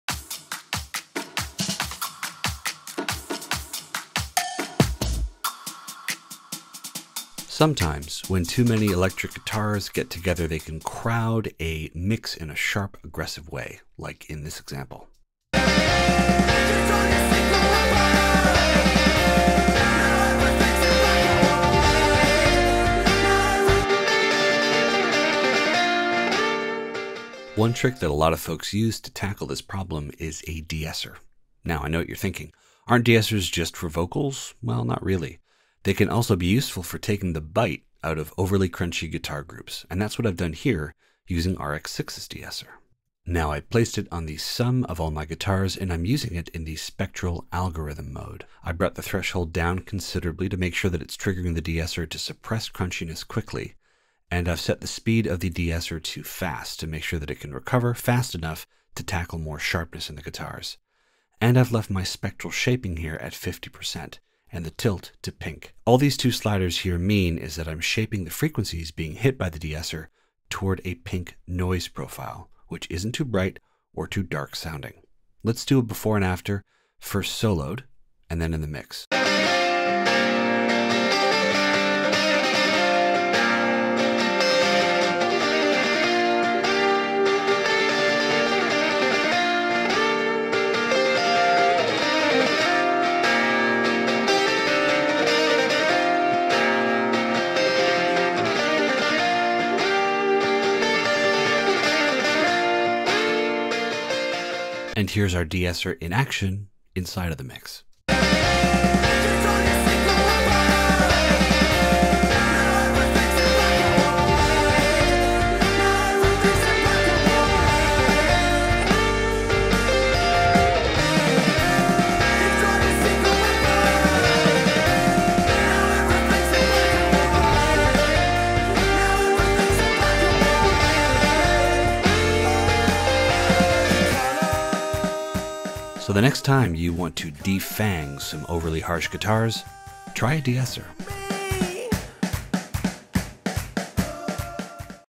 2-Minute Tips: Tame Sharp Guitar Sounds with a De-esser
When you have multiple guitar tracks overlapping, they can potentially make your mix sound crowded and unpleasantly sharp. Learn how to tame guitars with a de-esser.